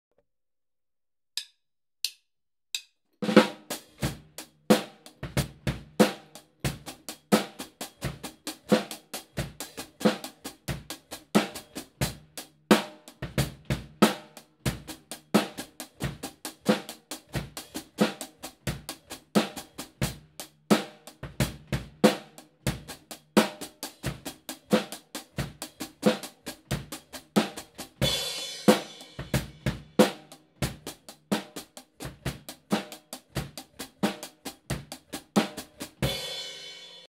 Manasseh ~ Para Diddle Hi Hat Triplets
im playing the closing of the hi hat as a note. then playing a para diddle with my right hand and left foot. once thats down i played it as 8th note triplets with the basic rock beat. this is the first draft i guess you could say of this groove. i will definitely embellish this groove once i get more comfortable with it. this is just the start. once you have all that down BAM! a cool sounding funky groove just pops out like a cute puppy waiting to be milked and and nurtured into a strong dog that can run and play with your kids (if you have any) lol two paradiddles (RLRR LRLL) play THREE times (RLRR LRLL RLRR LRLL RLRR LRLL ) in two measures of 8th note triplets on the hi hat. now if you play the groove on the hi hat as 8th notes there are just two para diddles (RLRR LRLL) hope that explanation made sense. share and comment please!!! and subscribe to ma channel.